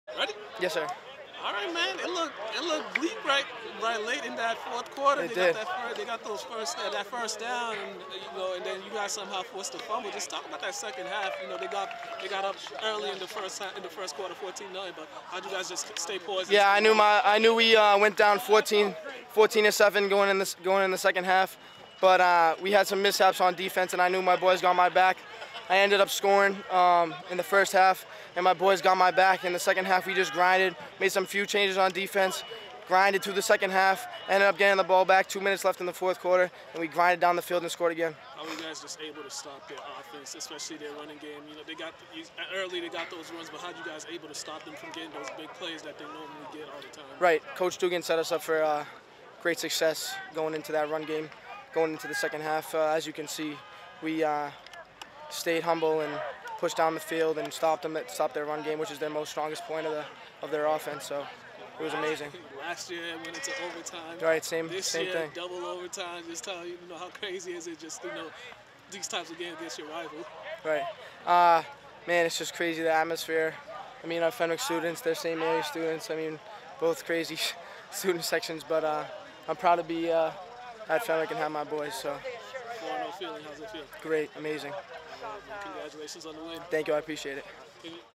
Costa Beechin postgame interview
Bishop Fenwick's, Costa Beechin after their double overtime win over St. Mary's Friday night.